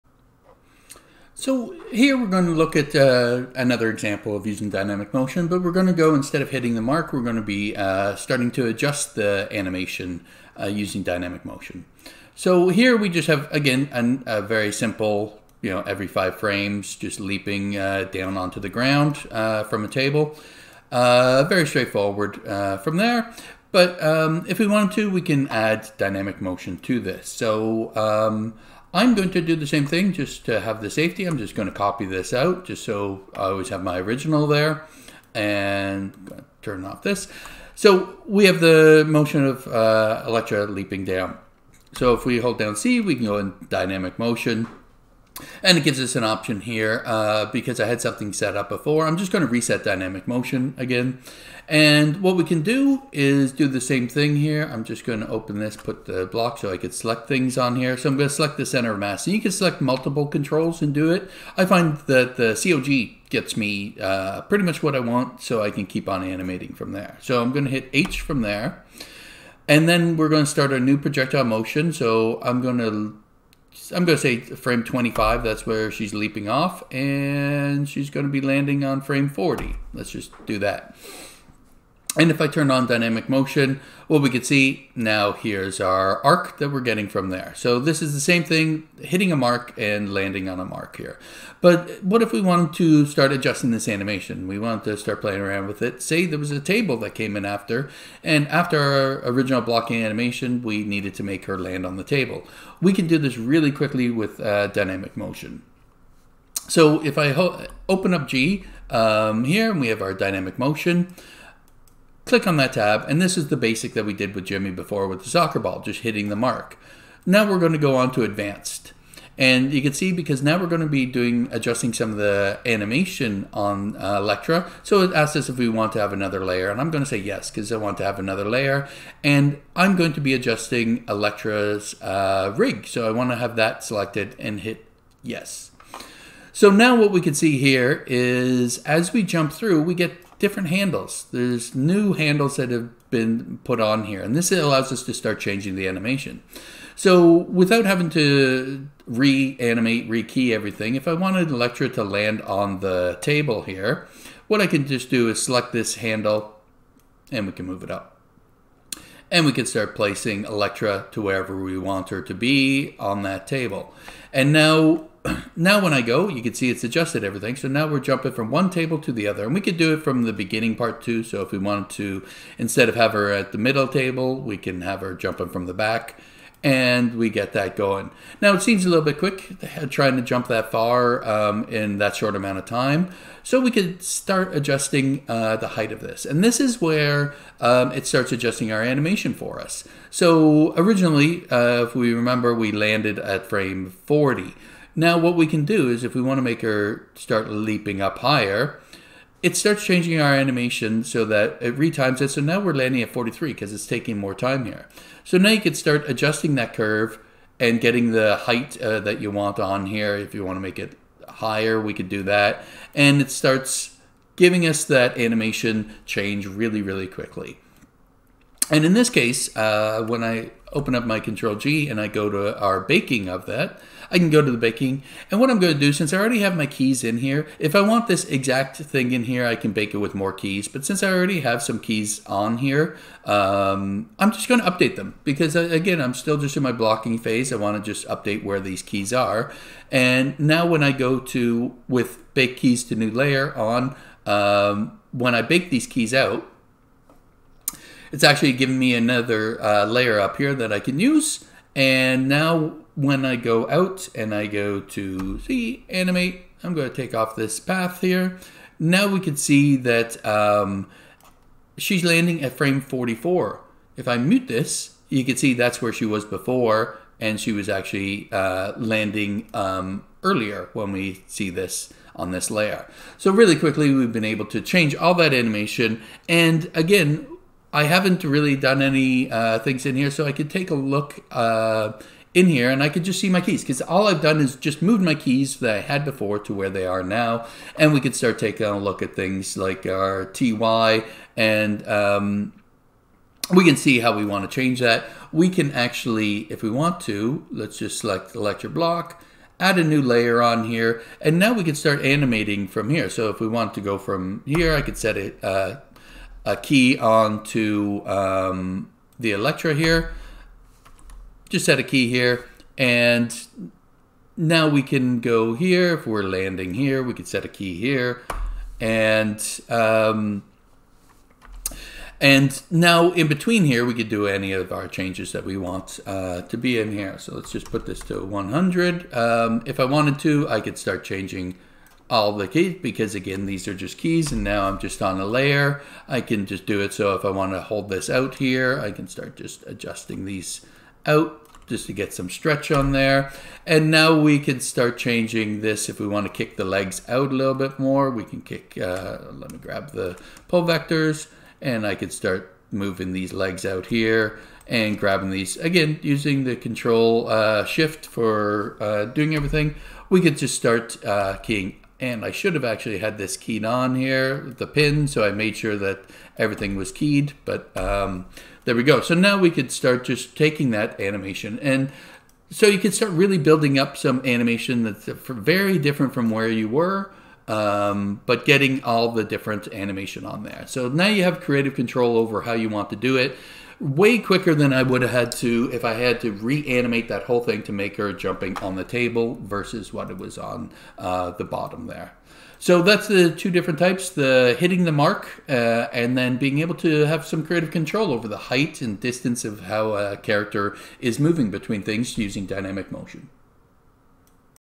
Animation Workshop 10 | Dynamic Motion | Adjust Animation
Use the Dynamic Motion advanced options to reposition, change the height of the motion and have your underlying animation keys be retimed to the new motion for you.